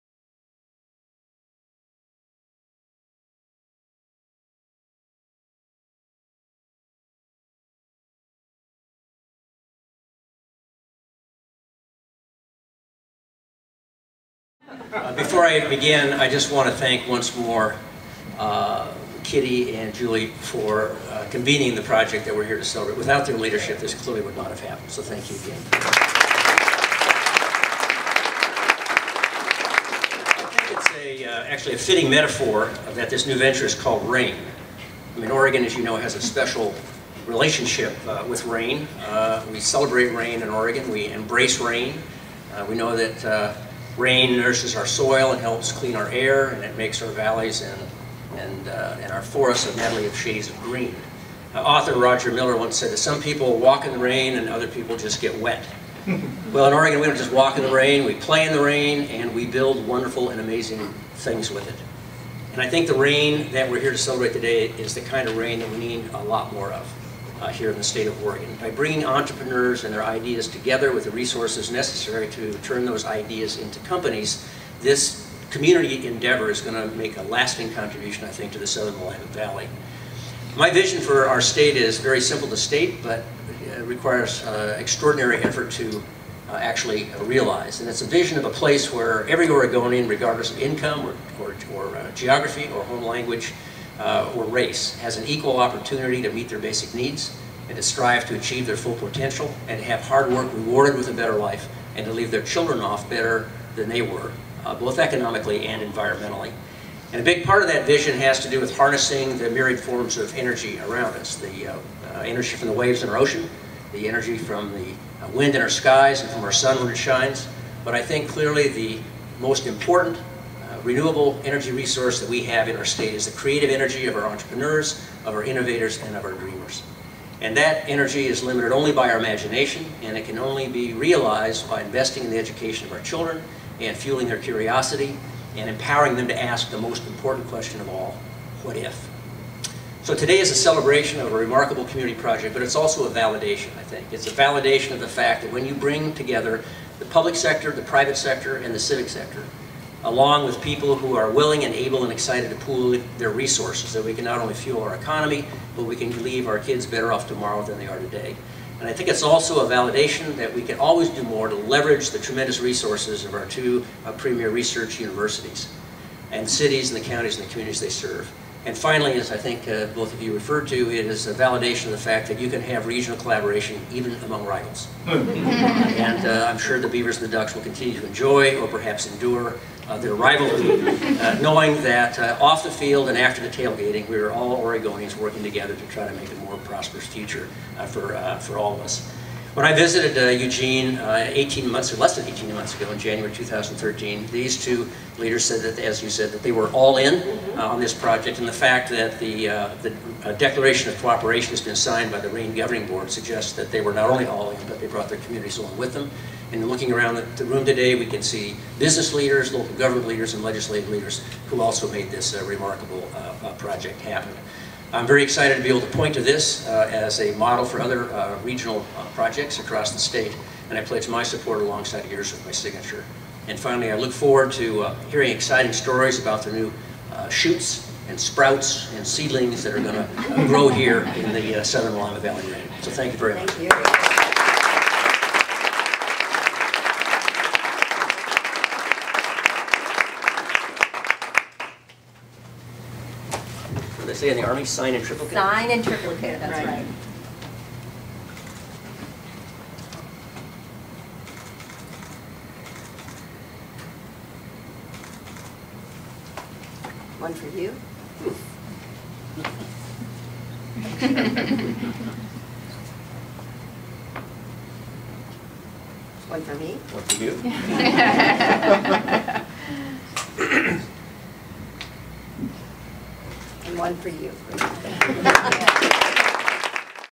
052814 RAIN Signing
Eugene, Oregon
Governor Kitzhaber signs a Declaration of Cooperation in support of the Regional Accelerator & Innovation Network (RAIN).